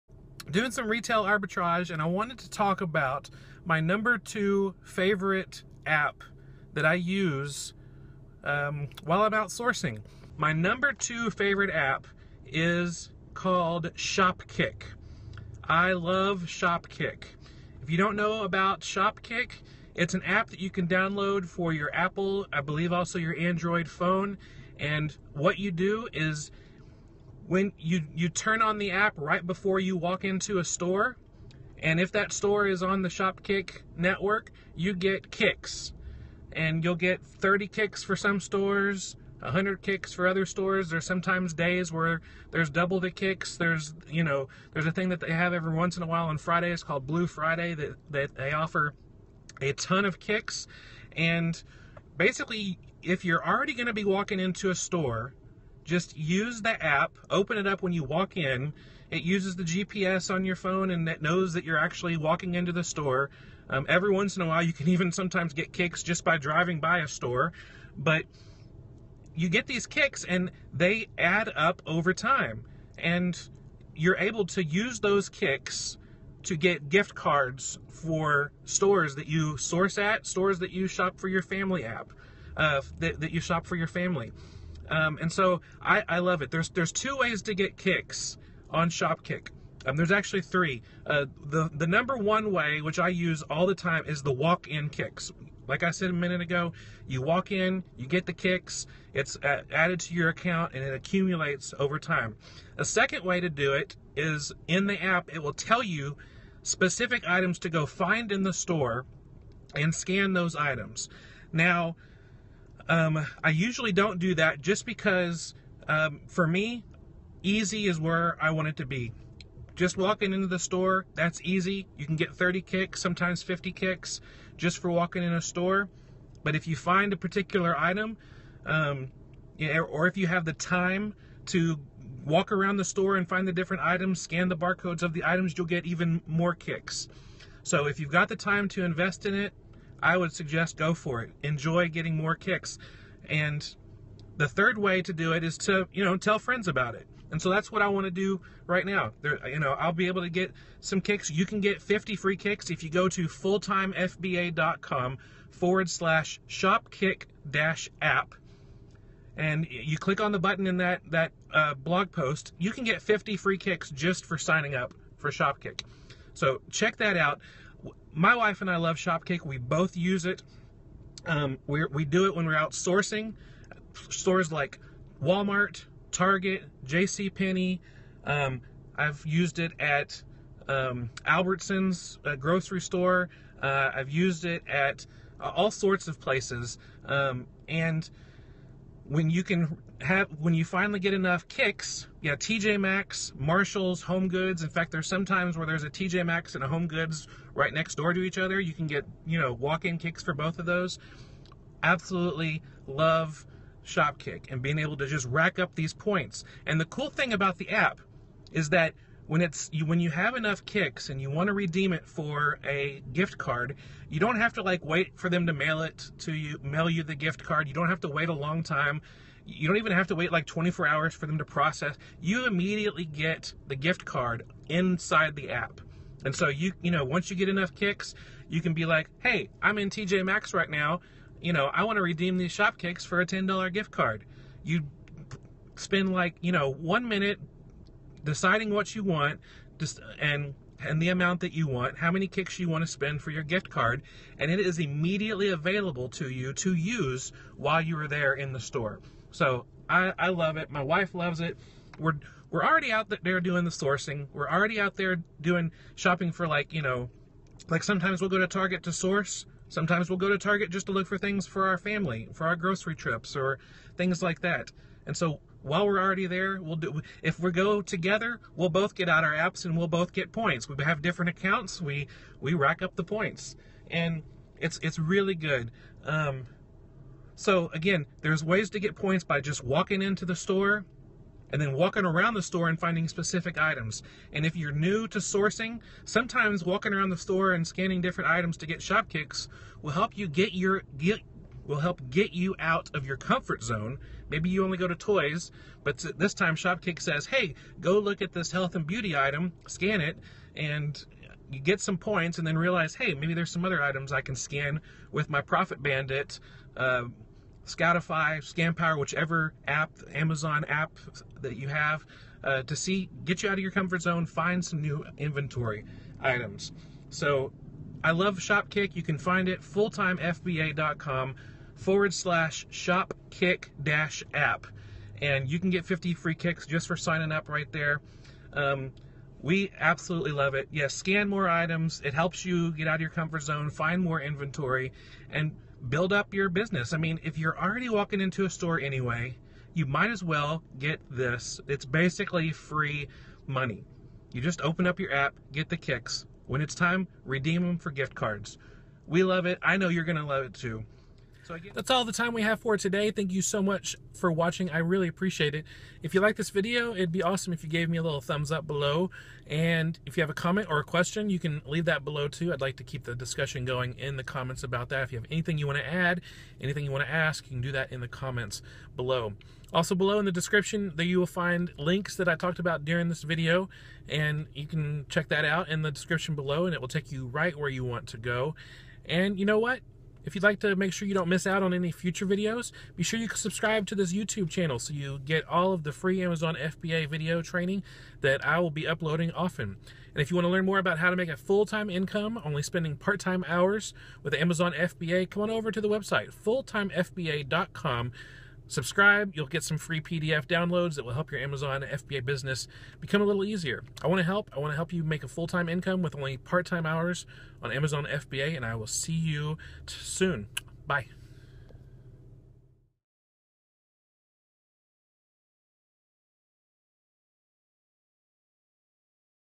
Retail Arbitrage Apps - Using Shopkick App While Sourcing Amazon FBA
Join Stephen for this Amazon FBA training video! Learn 3 ways we use the Shopkick app to earn gift cards for retail arbitrage sourcing.

Hope this video is helpful! Leave your questions and comments below!